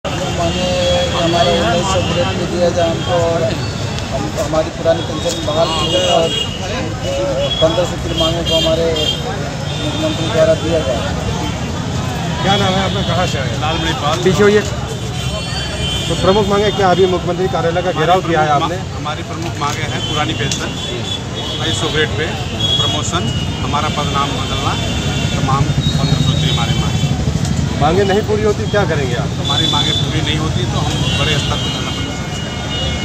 CM आवास से खदेड़े गए सफाई कर्मचारी, GPO पर जलाई नई पेंशन प्रतियां
लखनऊ : पुरानी पेंशन बहाली समेत अपनी 15 सूत्रीय मांगों के साथ पैदल वाराणसी से लखनऊ पहुंचे ग्रामीण  सफाई कर्मचारियों में सोमवार को मुख्यमंत्री आवास का घेराव किया. इस दौरान पुलिस बल ने सफाई कर्मचारियों को सीएम कार्यालय के बाहर रुकने नहीं दिया.